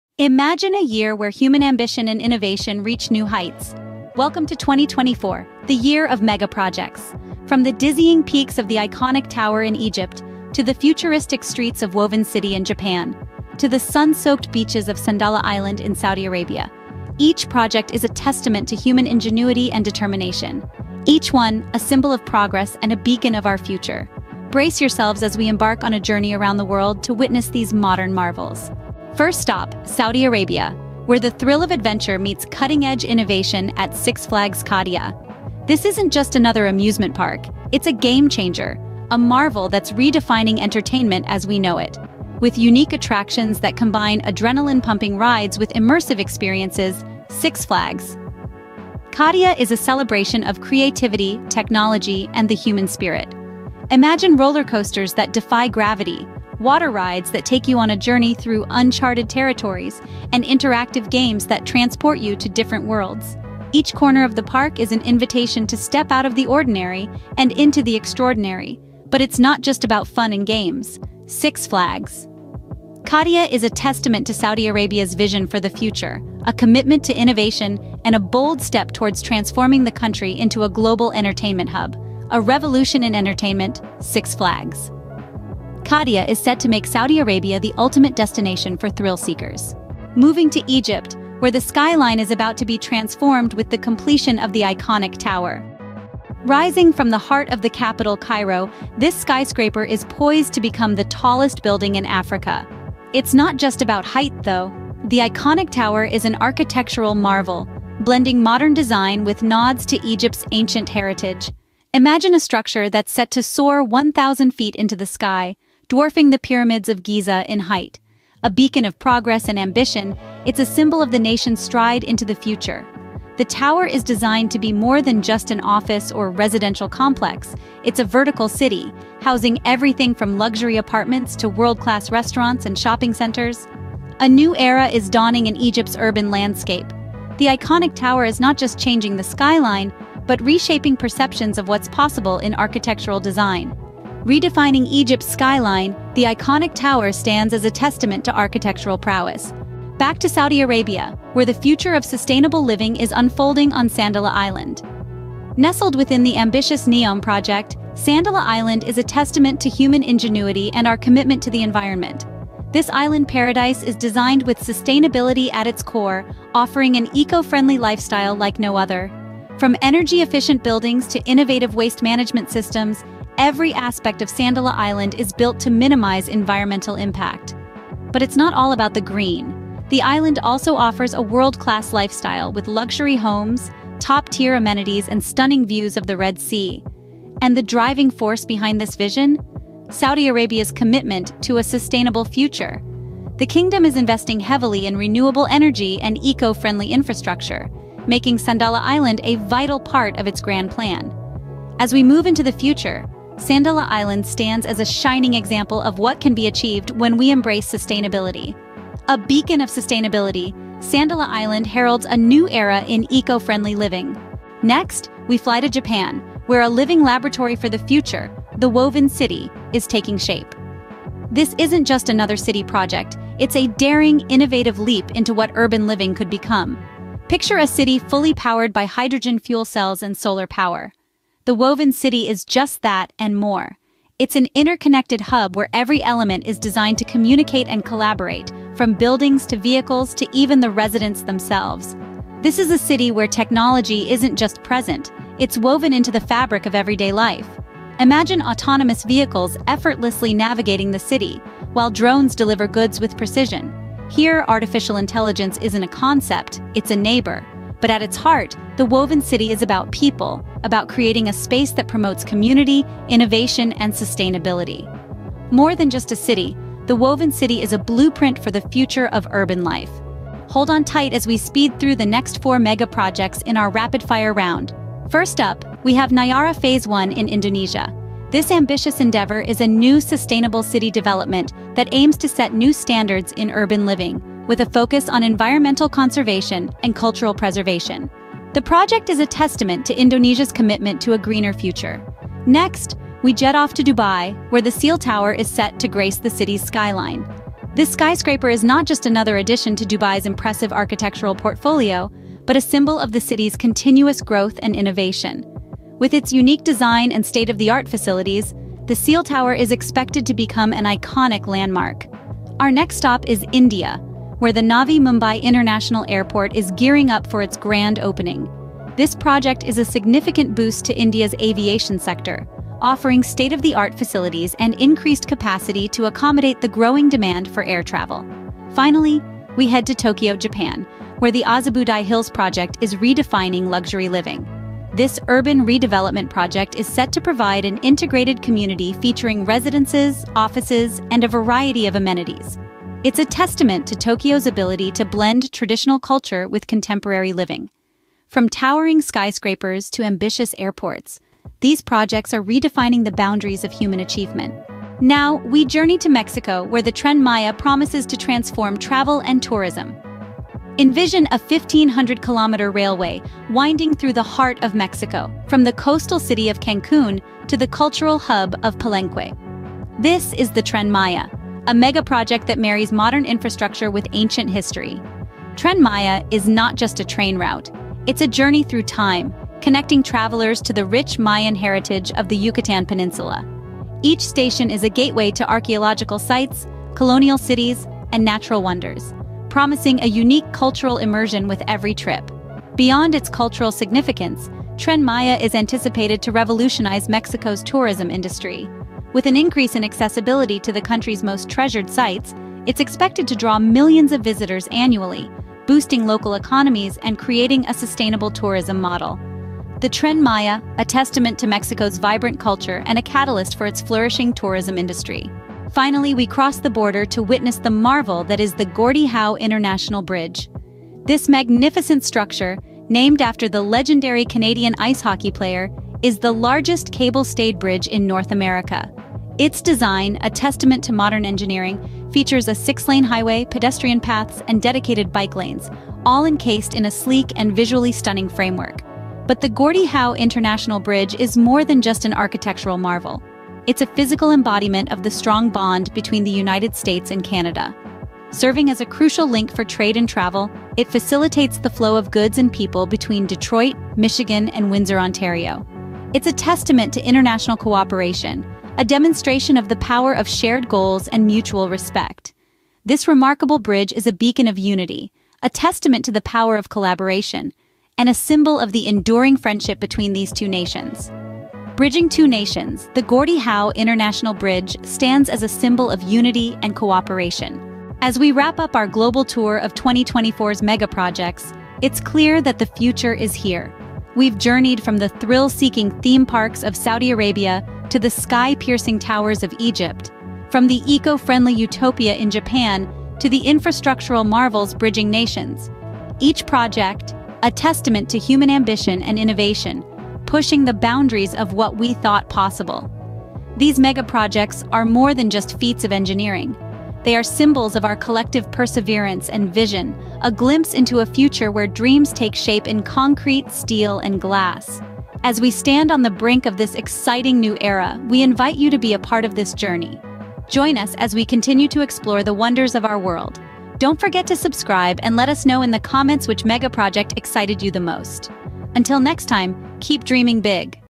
Top 10 Biggest Megaprojects Completing In 2024!
Get ready to be amazed by the Top 10 Biggest Megaprojects Completing in 2024! Join us on a global journey as we unveil the most ambitious and transformative projects set to redefine skylines and industries worldwide. From breathtaking infrastructure to cutting-edge technology, this video showcases the megastructures shaping our future.